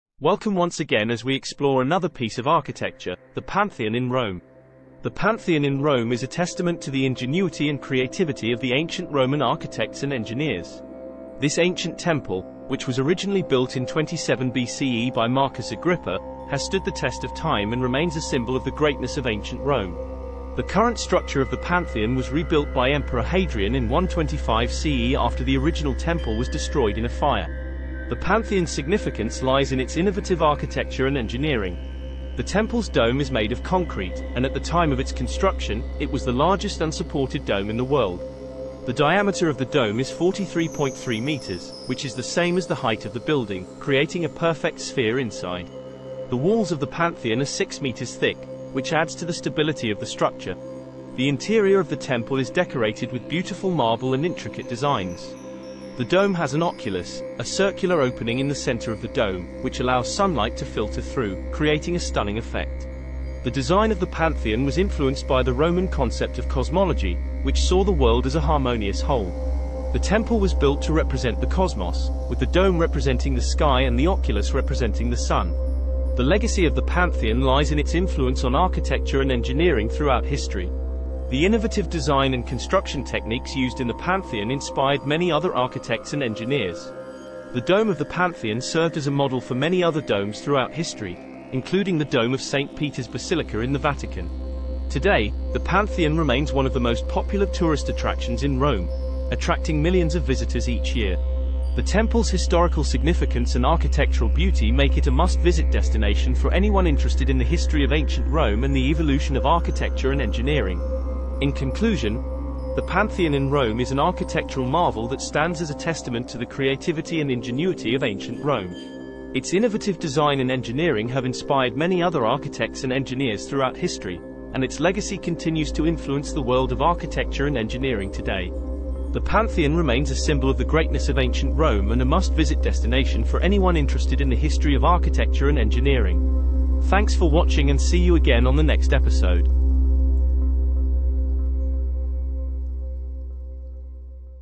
PANTHEON ROME || History of Architecture
PANTHEON ROME

The Pantheon in Rome is a testament to the ingenuity and creativity of the ancient Roman architects and engineers. This ancient temple, which was originally built in 27 BCE by Marcus Agrippa, has stood the test of time and remains a symbol of the greatness of ancient Rome.
The current structure of the Pantheon was rebuilt by Emperor Hadrian in 125 CE after the original temple was destroyed in a fire. The Pantheon's significance lies in its innovative architecture and engineering. The temple's dome is made of concrete, and at the time of its construction, it was the largest unsupported dome in the world. The diameter of the dome is 43.3 meters, which is the same as the height of the building, creating a perfect sphere inside.
The walls of the Pantheon are 6 meters thick, which adds to the stability of the structure. The interior of the temple is decorated with beautiful marble and intricate designs. The dome has an oculus, a circular opening in the center of the dome, which allows sunlight to filter through, creating a stunning effect.
The design of the Pantheon was influenced by the Roman concept of cosmology, which saw the world as a harmonious whole. The temple was built to represent the cosmos, with the dome representing the sky and the oculus representing the sun.
The legacy of the Pantheon lies in its influence on architecture and engineering throughout history. The innovative design and construction techniques used in the Pantheon inspired many other architects and engineers. The dome of the Pantheon served as a model for many other domes throughout history, including the dome of St. Peter's Basilica in the Vatican.
Today, the Pantheon remains one of the most popular tourist attractions in Rome, attracting millions of visitors each year. The temple's historical significance and architectural beauty make it a must-visit destination for anyone interested in the history of ancient Rome and the evolution of architecture and engineering.
In conclusion, the Pantheon in Rome is an architectural marvel that stands as a testament to the creativity and ingenuity of ancient Rome. Its innovative design and engineering have inspired many other architects and engineers throughout history, and its legacy continues to influence the world of architecture and engineering today. The Pantheon remains a symbol of the greatness of ancient Rome and a must-visit destination for anyone interested in the history of architecture and engineering.